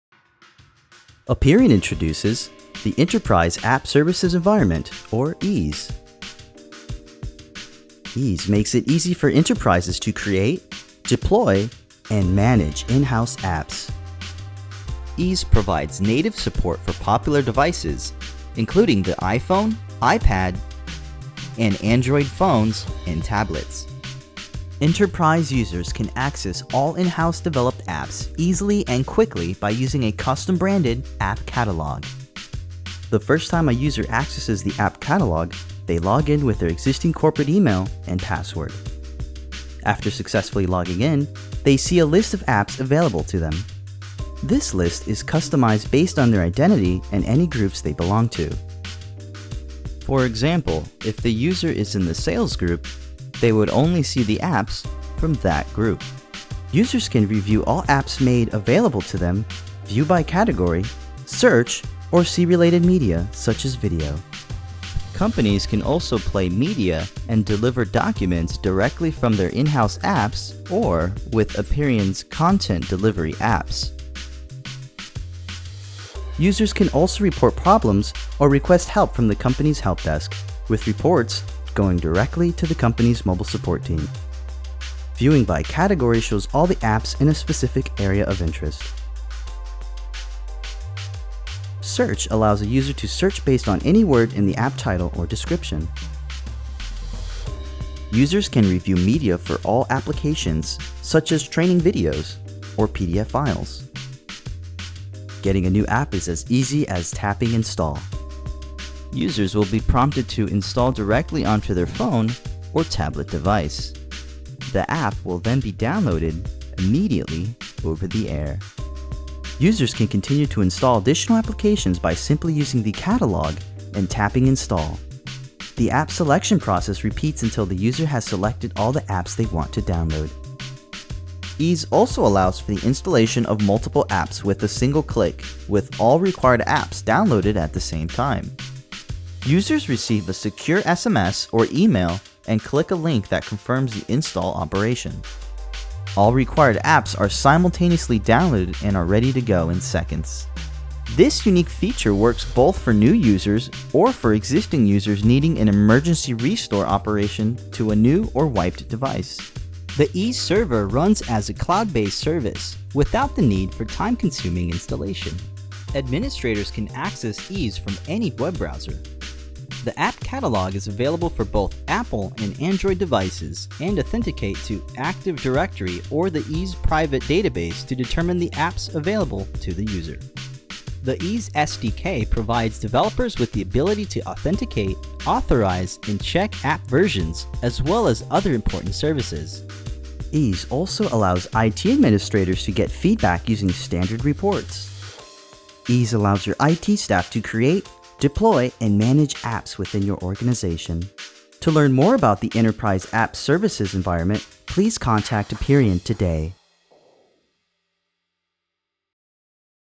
iOS and Android Enterprise App Services Environment (EASE) by Apperian
Apperian's EASE (Enterprise App Services Environment) technology provides enterprises with the capability to distribute, update, manage, and provision in-house iOS and Android apps to an entire organization.

This demo shows the user experience in viewing and installing Apps on mobile phones and tablets from the "Enterprise App Catalog". See multiple apps installed over-the-air with a single click!

Enterprise IT has the control needed in large organizations, while providing users with a familiar application interface from which to choose applications, view additional application information, and access app-related training materials.

Apple and Android Enterprise Developers can create in-house applications for iOS and Android mobile phones and tablets. In addition, EASE provides standard developers with a great way to manage ad-hoc delivery of apps to customers and beta testers during the development process.